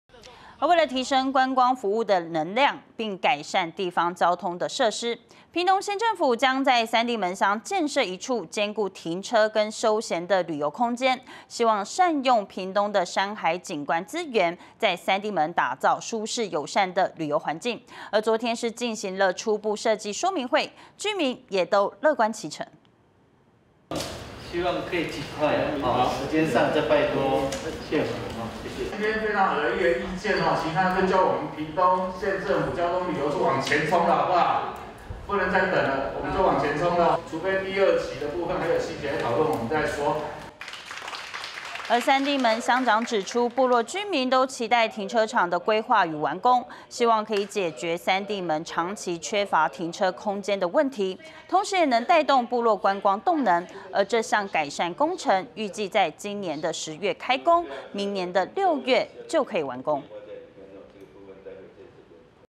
縣府將建停車兼休閒空間 三地門居民樂見其成｜每日熱點新聞｜原住民族電視台
為了提升觀光服務能量，並改善地方交通設施，屏東縣政府將在三地門鄉建設一處兼顧停車跟休閒的旅遊空間，希望善用屏東的山海景觀資源，在三地門打造舒適友善的旅遊環境，昨天(9)進行初步設計說明會，居民都樂見其成。